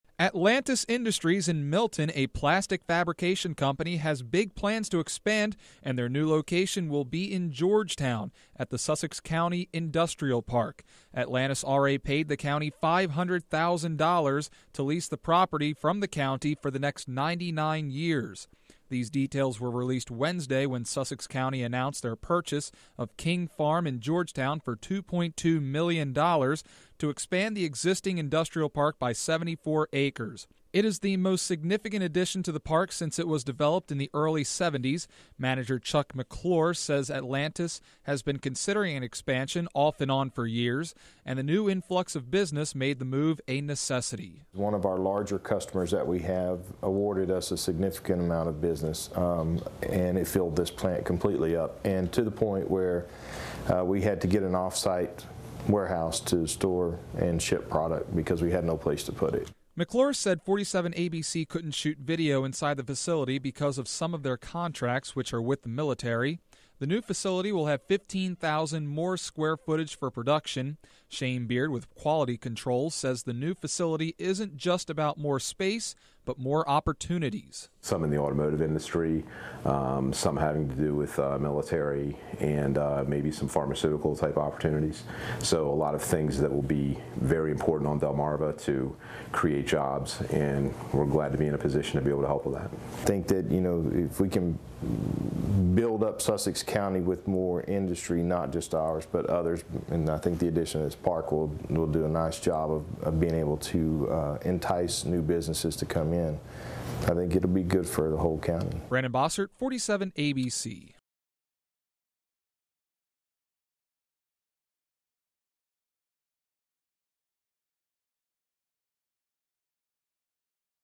Atlantis Industries moves to Sussex Industrial Park - 47ABC
I worked with Brandon Bossert visiting Atlantis Industries in Georgetown, discussing their move to a bigger plant.